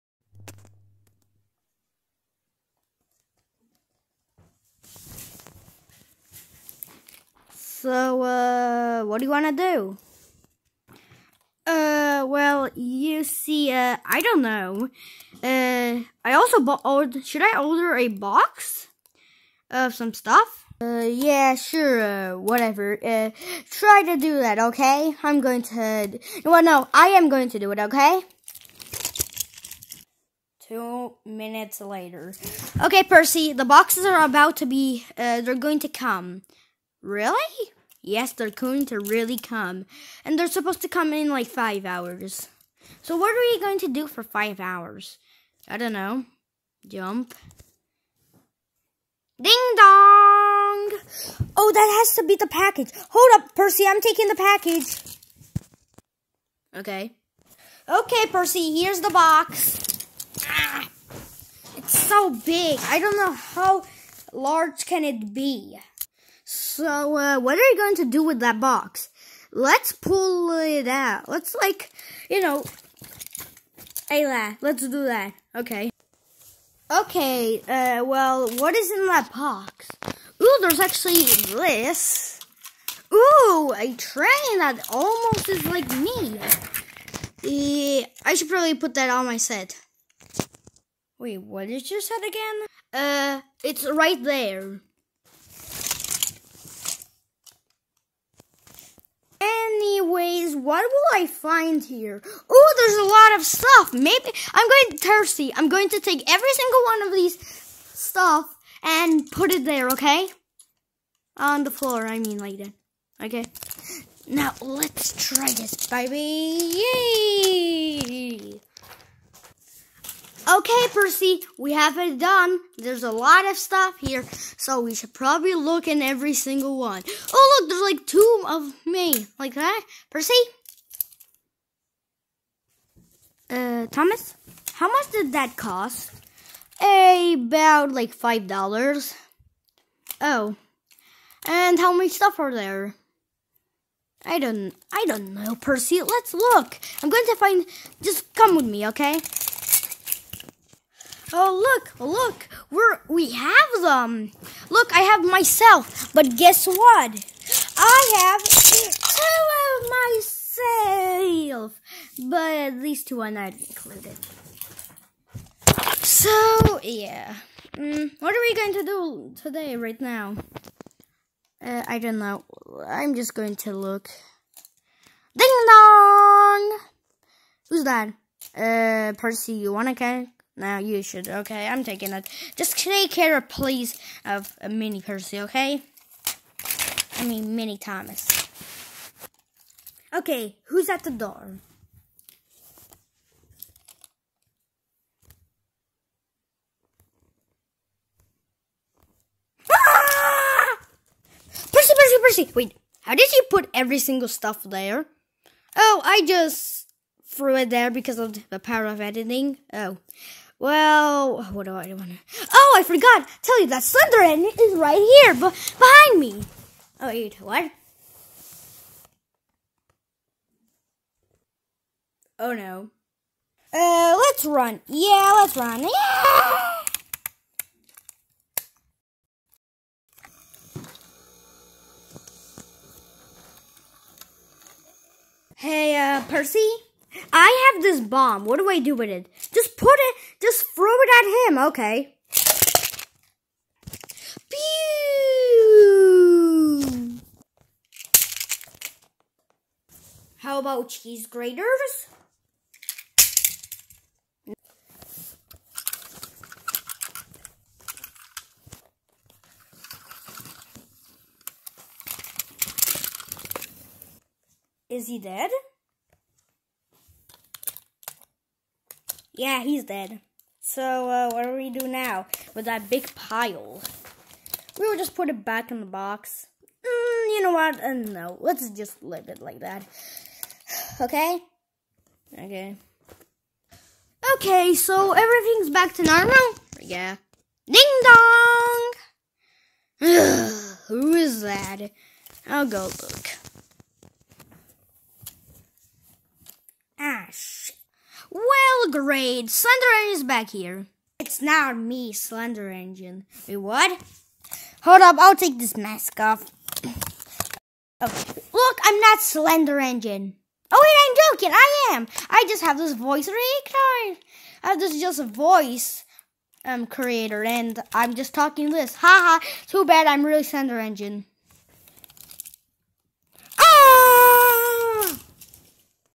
Thomas parody: S1 E2 The slender tank engine.
The slender tank engine came to Thomas's home's.